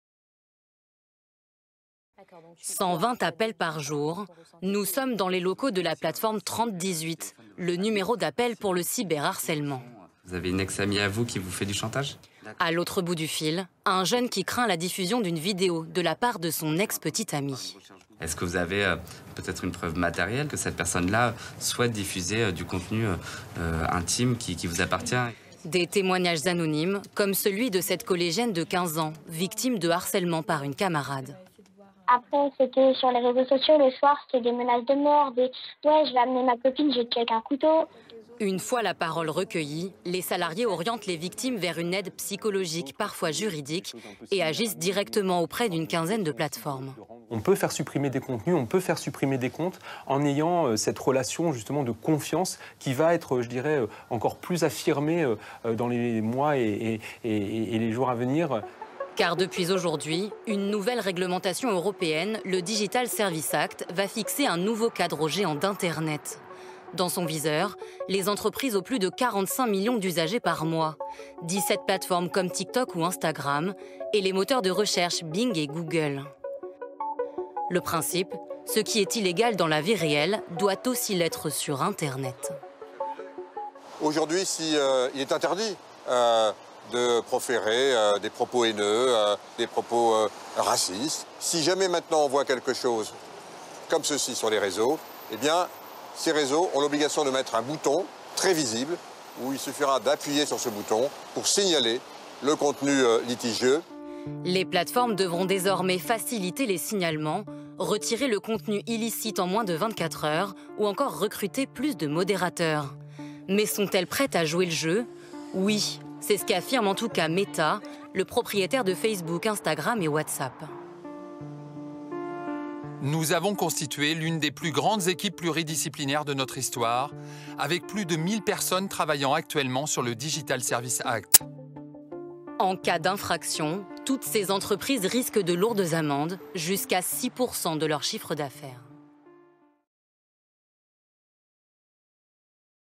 📜 Une nouvelle réglementation européenne pour protéger les utilisateurs sur les Réseaux sociaux
Le "Digital services act" est entré en vigueur, vendredi 25 août. Cette nouvelle réglementation européenne donne un nouveau cadre légal aux géants d'Internet, et vise à mieux lutter contre le harcèlement en ligne, entre autres objectifs.

La plateforme 30 18, le numéro d'appel pour le cyberharcèlement, reçoit 120 appels par jour. À l'autre bout du fil, ce jour-là, un jeune craint la diffusion d'une vidéo de la part de son ex-petit ami. Parmi les témoignages anonymes, on trouve celui d'une collégienne de 15 ans, victime de harcèlement par une camarade. Une fois la parole recueillie, les salariés orientent les victimes vers une aide psychologique, parfois juridique, et agissent directement auprès d'une quinzaine de plateformes.

Un nouveau cadre pour les géants d'Internet
"On peut faire supprimer des contenus, on peut faire supprimer des comptes, en ayant cette relation de confiance, qui va être encore plus affirmée dans les mois et les jours à venir", explique Samuel Comblez, directeur des opérations du 30 18. Depuis vendredi 25 août, une nouvelle réglementation européenne, le "Digital services act", va en effet fixer un nouveau cadre aux géants d'Internet. Il a dans son viseur les entreprises aux plus de 45 millions d'usagers par mois. Le principe est que ce qui est illégal dans la vie réelle doit aussi l'être sur Internet.